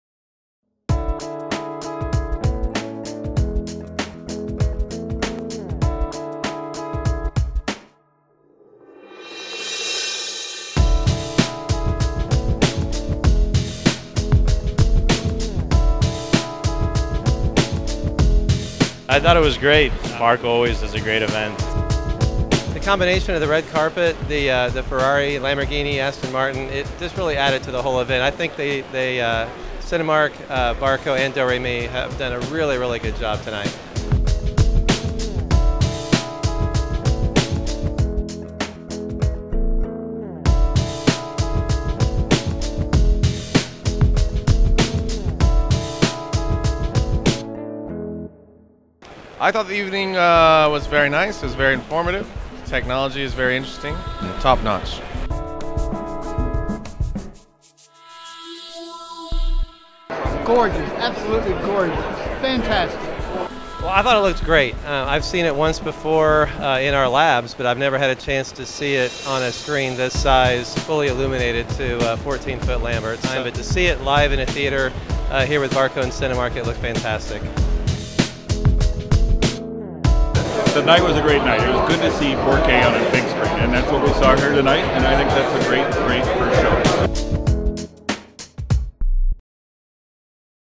Barco unveils brand-new 4K cinema projector
- On October 12th, Barco unveiled a brand-new DLP Cinema® Enhanced 4K projector at a special red carpet event in Cinemark's Festival Bay Theatre in Orlando, USA. The new 4K projector complements Barco's current 2K projector offering, and will become commercially available in early 2011.